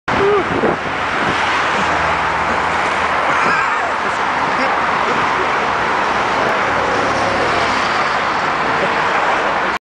Stuck in Moscow Traffic
Kon and Metzger get caught in traffic for 15 minutes with cars flying by at 50 mph